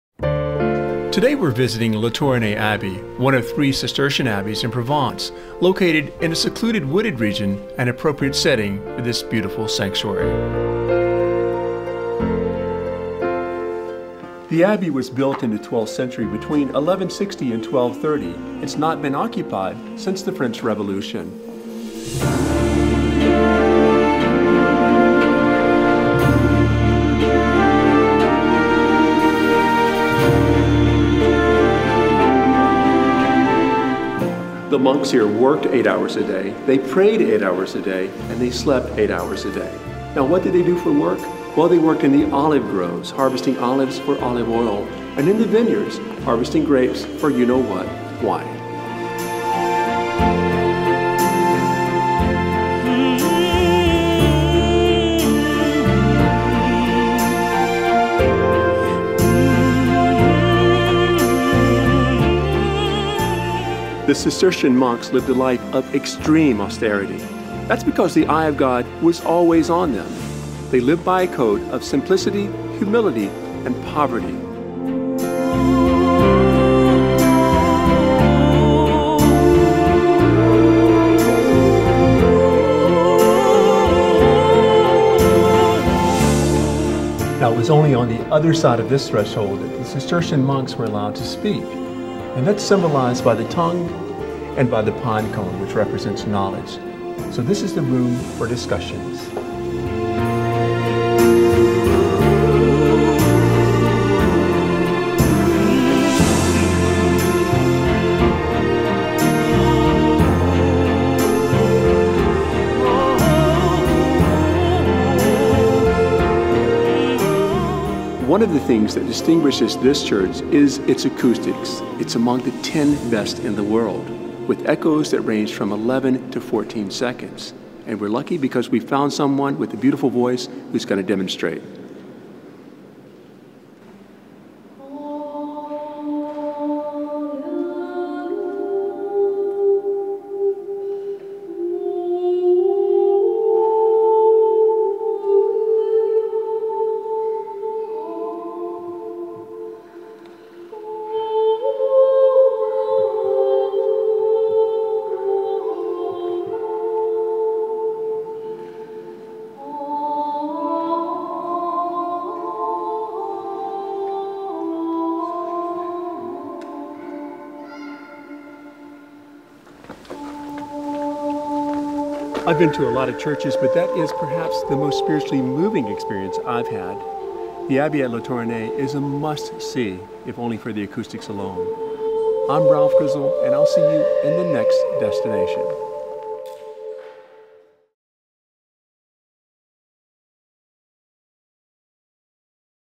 The Avid Cruiser Takes On Le Thoronet Abbey
The Avid Cruiser Takes On Le Thoronet Abbey from The Avid Cruiser.

The Avid Cruiser explores the beautiful Cistercian Thoronet Abbey in the Var region of southern France.